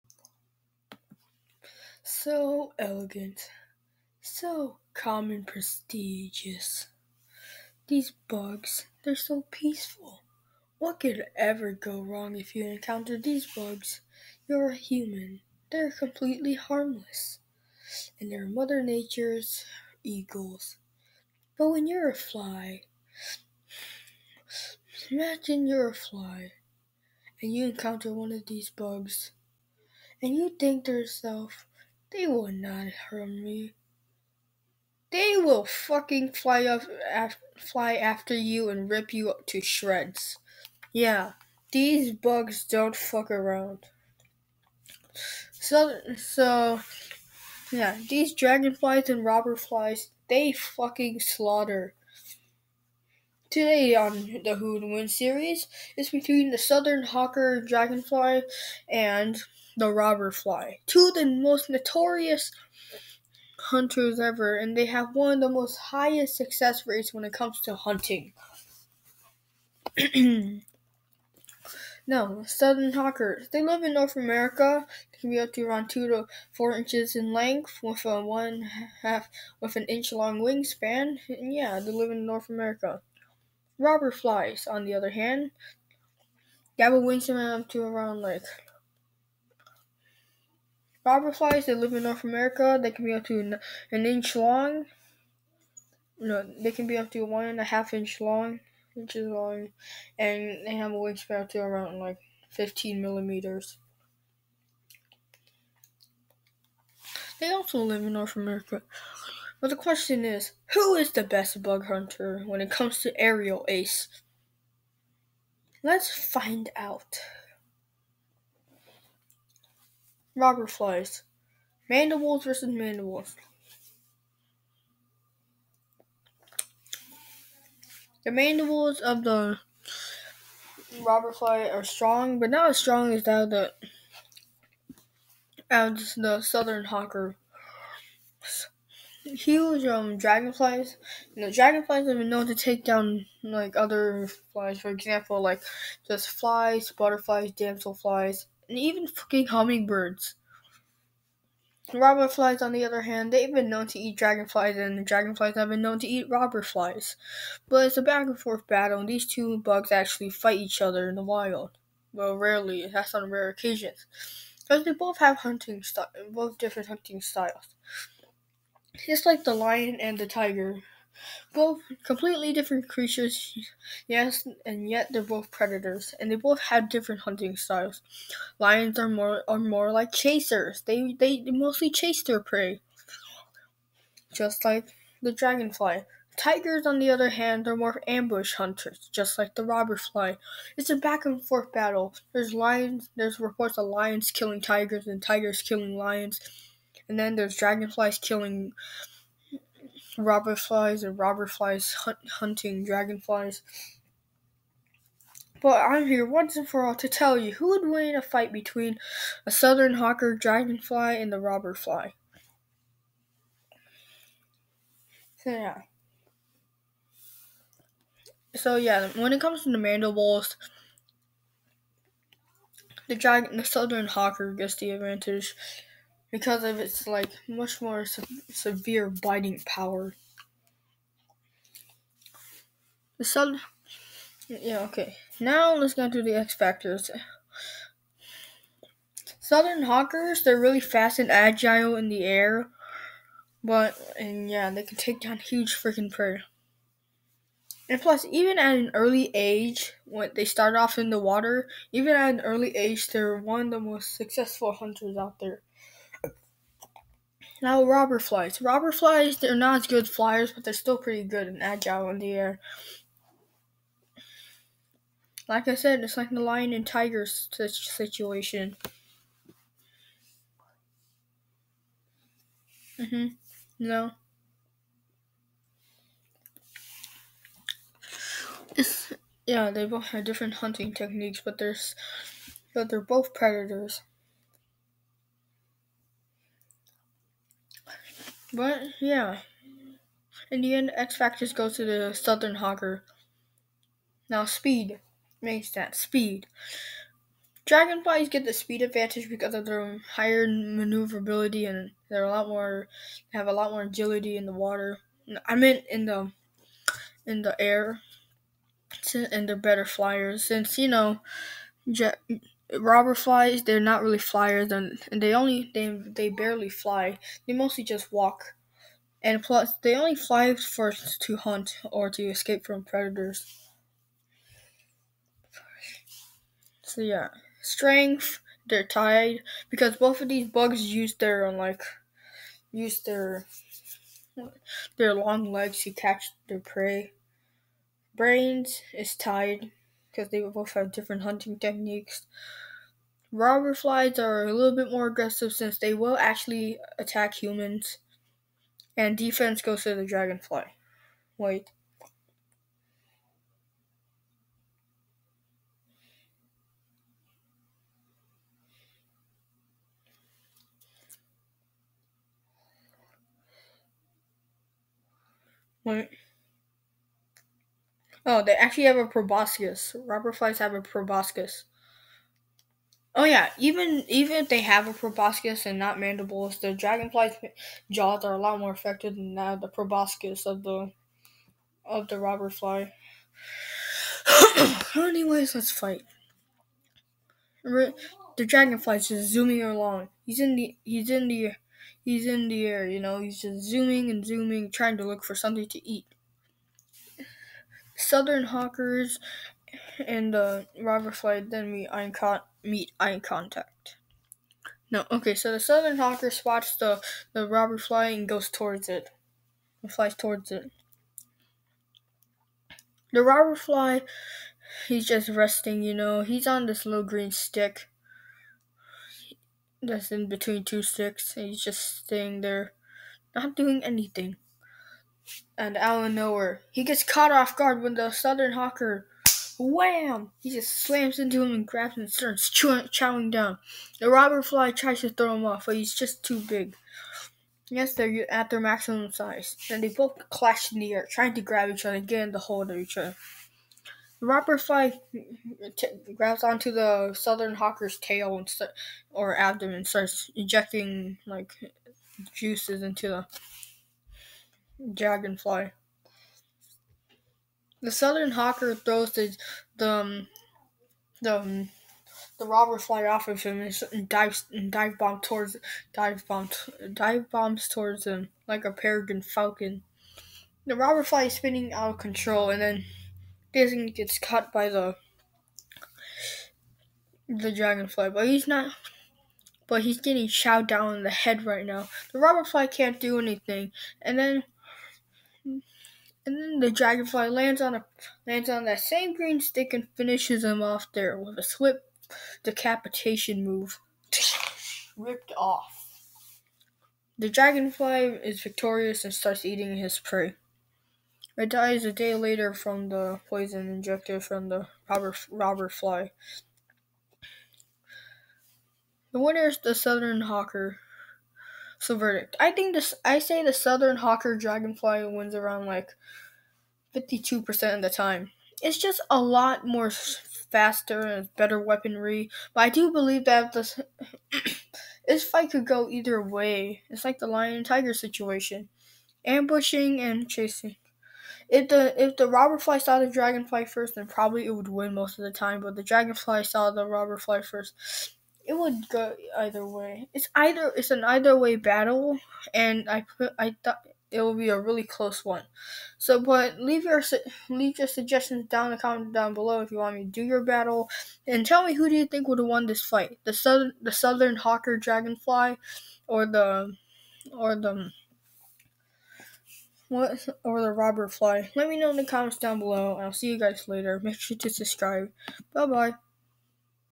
Southern Hawker Dragonfly vs Robber Fly
2 underestimated bugs that are actually really good hunters fight each other for food.